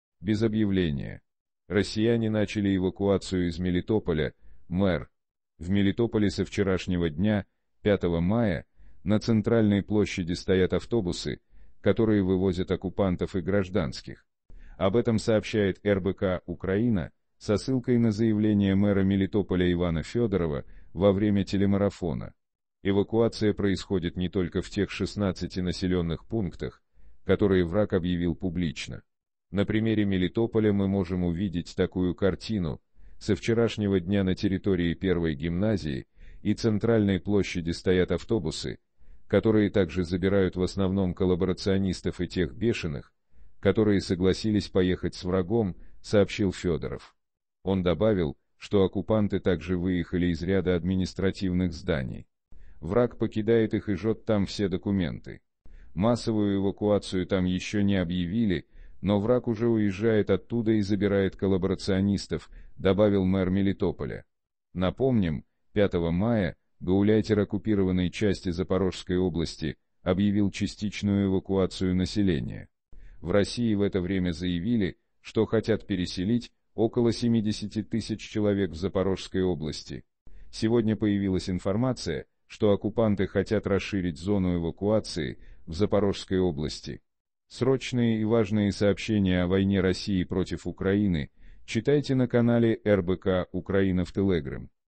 Без объявления. Россияне начали "эвакуацию" из Мелитополя, - мэр.
Мелитополь ВойнаРоссиипротивУкраины В Мелитополе со вчерашнего дня, 5 мая, на центральной площади стоят автобусы, которые вывозят оккупантов и гражданских. Об этом сообщает РБК-Украина со ссылкой на заявление мэра Мелитополя Ивана Федорова во время телемарафона. "Эвакуация" происходит не только в тех 16 населенных пунктах, которые враг объявил публично. На примере Мелитополя мы можем увидеть такую картину: со вчерашнего дня на территории первой гимназии и центральной площади стоят автобусы, которые также забирают в основном коллабор